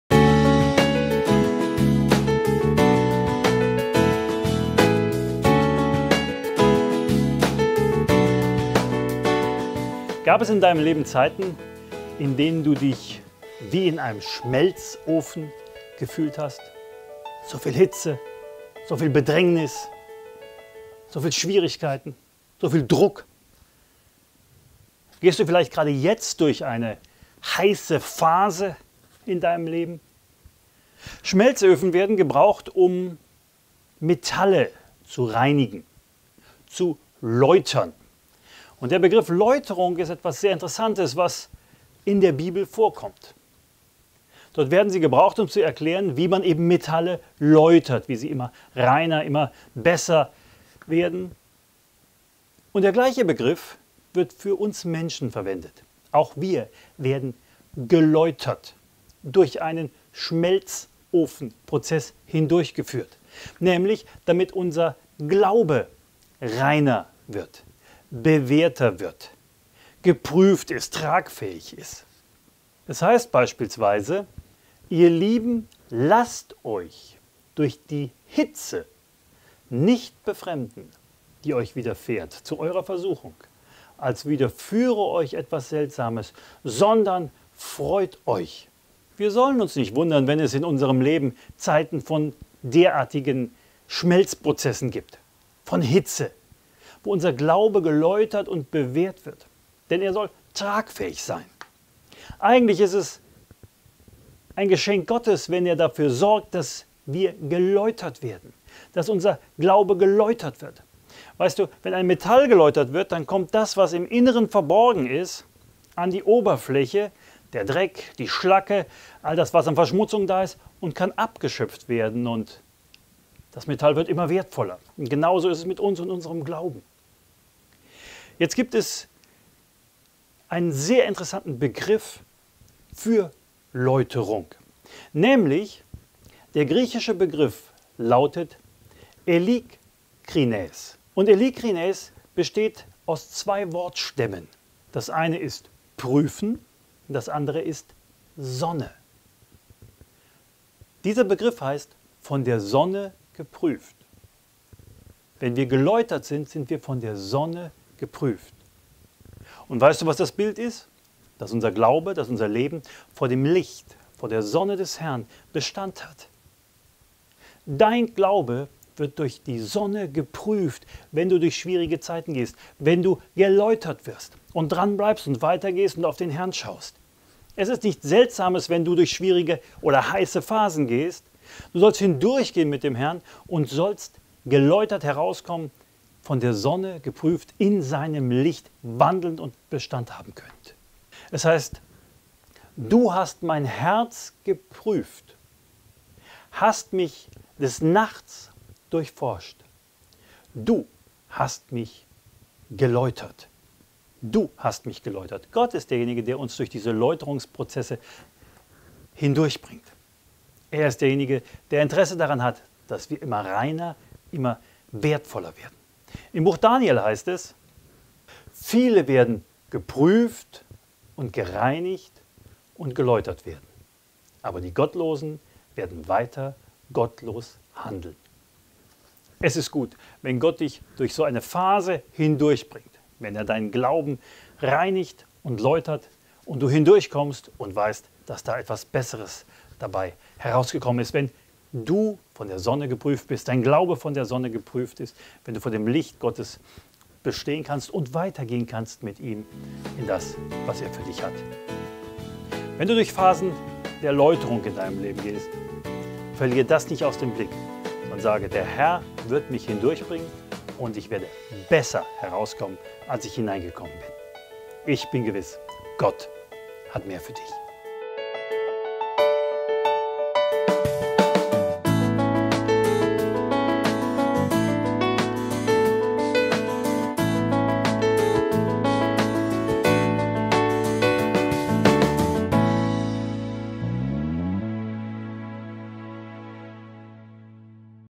Von der Sonne geprüft
Gab es in deinem Leben Zeiten, in denen du dich wie in einem Schmelzofen gefühlt hast? Gehst du vielleicht sogar gerade jetzt durch eine „heiße Phase"? Schmelzöfen werden benötigt, um Metalle zu reinigen und zu läutern -- doch die Bibel verwendet dieses Bild auch für Menschen, deren Glaube durch Prüfungen geläutert wird.

Unser Glaube soll sich als rein, standhaft, bewährt und tragfähig erweisen. Deshalb lässt Gott Zeiten zu, in denen wir geläutert werden:

Ihr Lieben, lasst euch durch die Hitze nicht befremden, die euch widerfährt zu eurer Versuchung, als widerführe euch etwas Seltsames, sondern freut euch...
1.Petrus 4,12-13

Der griechische Begriff des Wortes Läuterung lautet „eilikrinäs". Er ist aus den Wortstämmen „Sonne" und „prüfen" zusammengesetzt und bedeutet: „Von der Sonne geprüft".

Geläutert ist demnach ein Mensch, dessen Leben im hellen Licht der Sonne bestehen kann. Wenn du also gerade durch einen Prozess der Läuterung gehst, dann deshalb, weil Gott will, dass du der Prüfung seines Lichtes standhältst. Er will  dich auf ein ganz neues Level bringen.

Du hast mein Herz geprüft, hast mich des Nachts durchforscht; du hast mich geläutert
Psalm 17,3

Viele werden geprüft und gereinigt und geläutert werden. Aber die Gottlosen werden weiter gottlos handeln.
Daniel 12,10